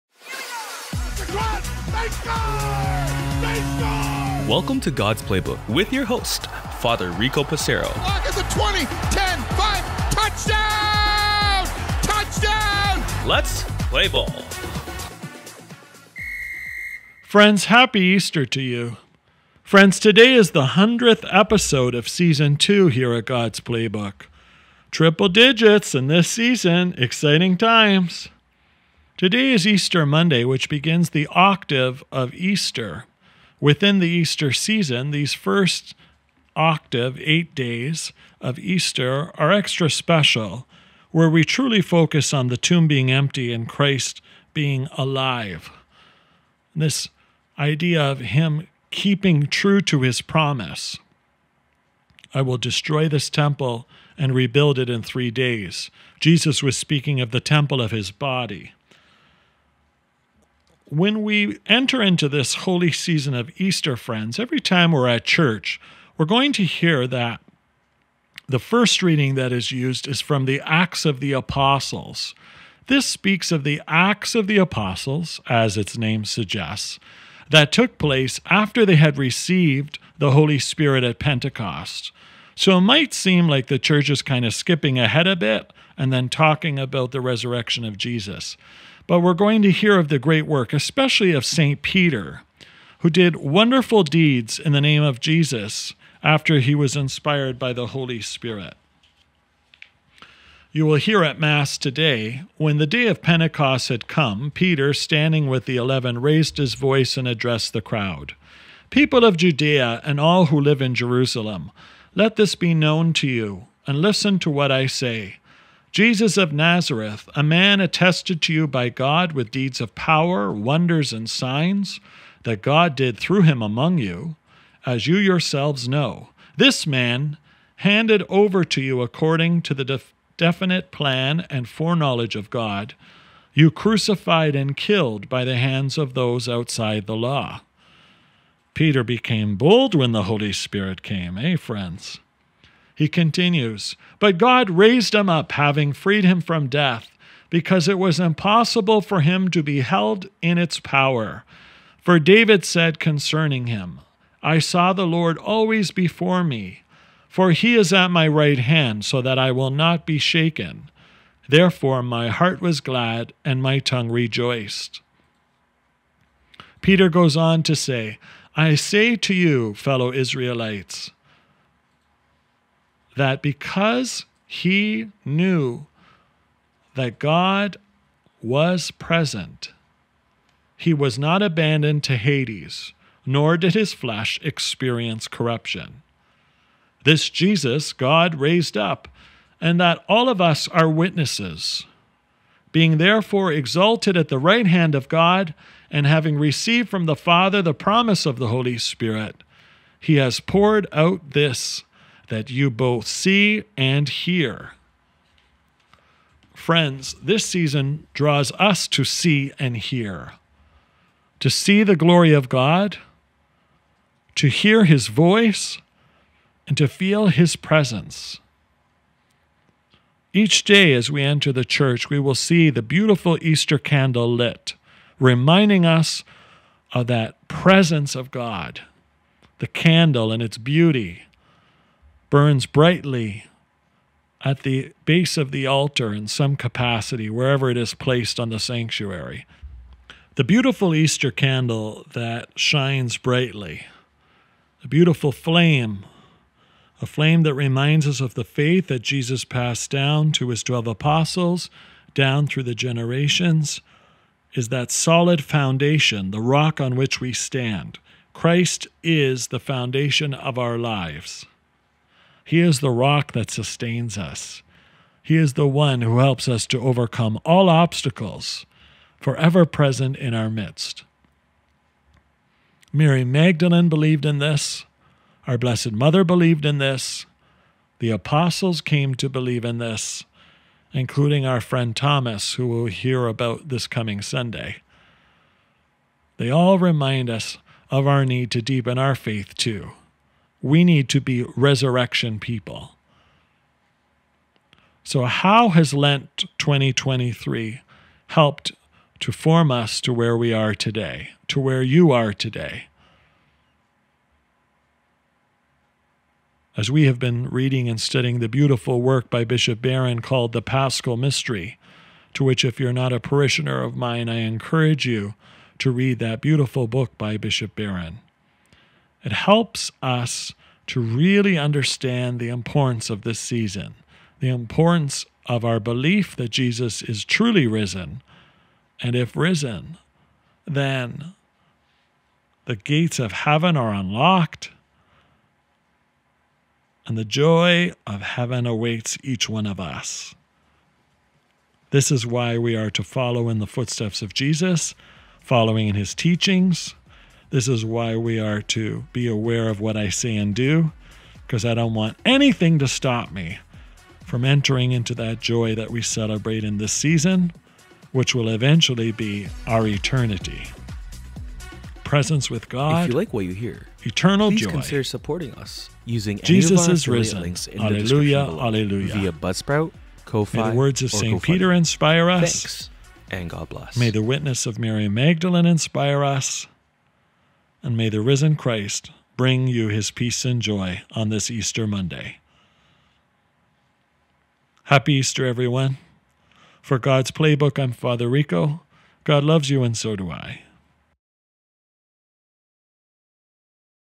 God's Playbook : S2 Episode 100 - Easter Monday
Today begins the octave of easter. These next 8 days are special during the octave of easter as our focus is on the tomb being empty and Jesus rising from the dead. During this season of Easter, we hear a lot from the Acts of the Apostles.  We also hear a lot of the works of St. Peter as he was inspired by the Holy Spirit. Let's join Fr. Rico and listen in on today's podcast episode.